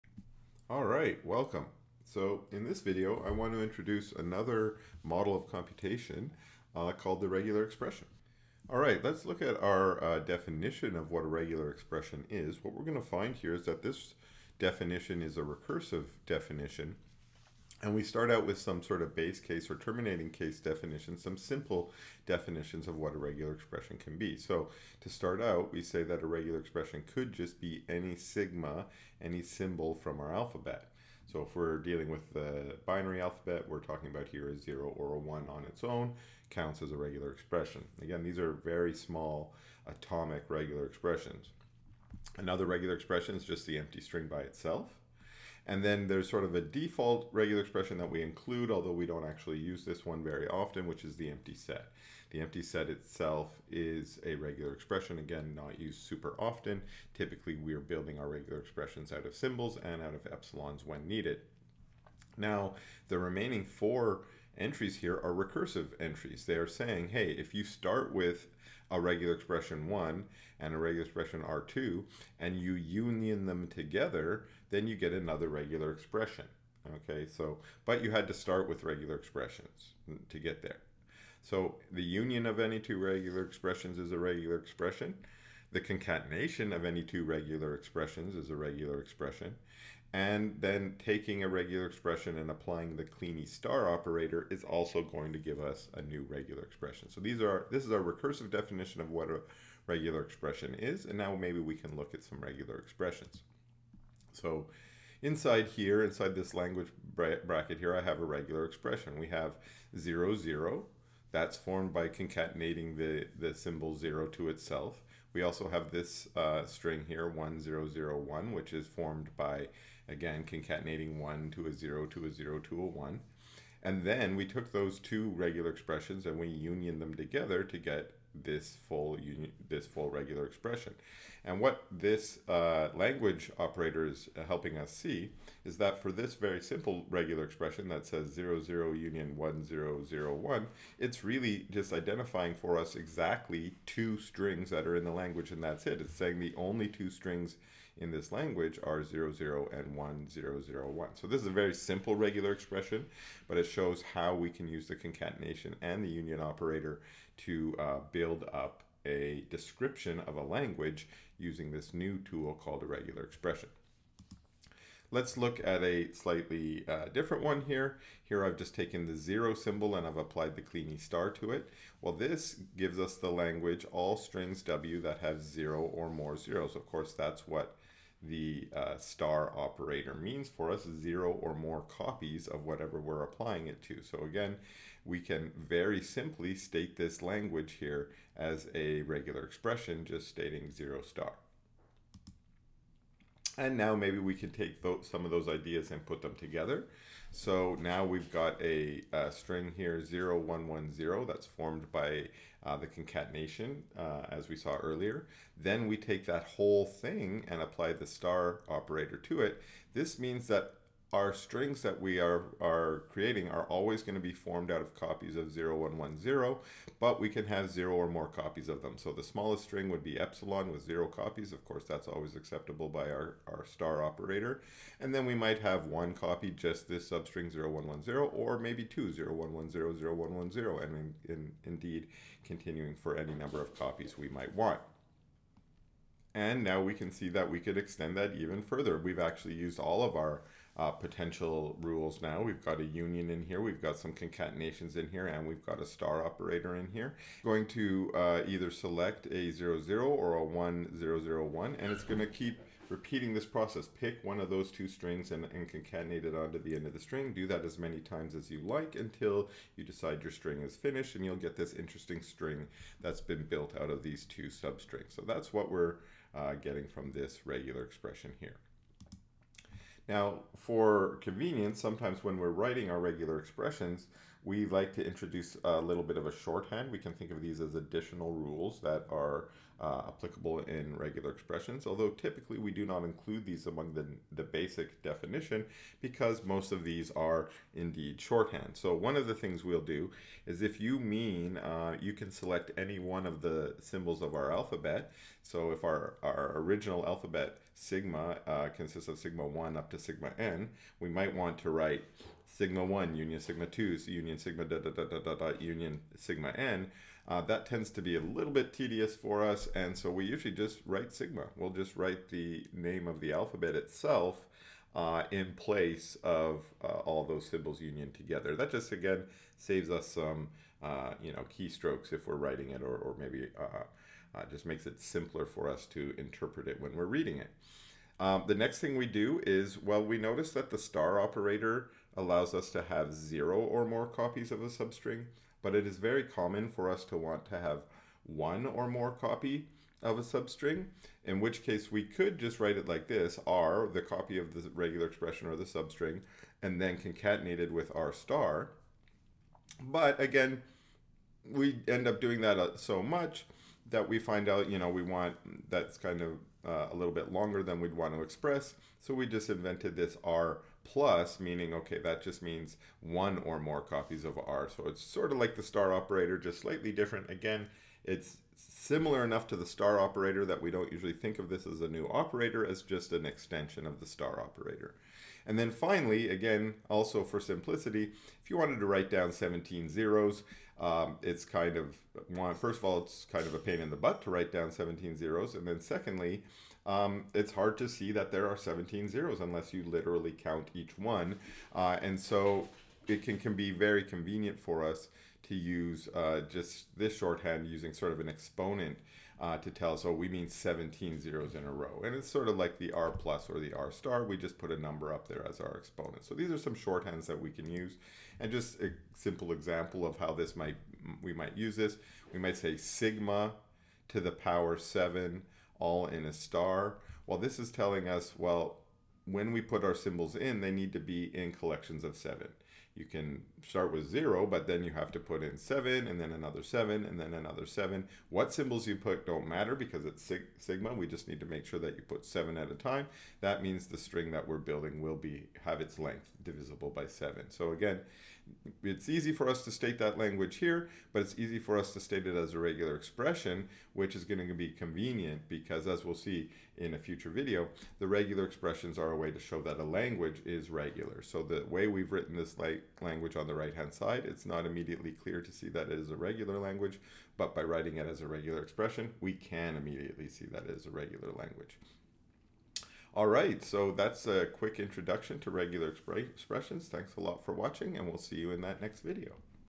Regular Expressions - Theory of Computing
In this video I introduce formal regular expressions as our third model of computation.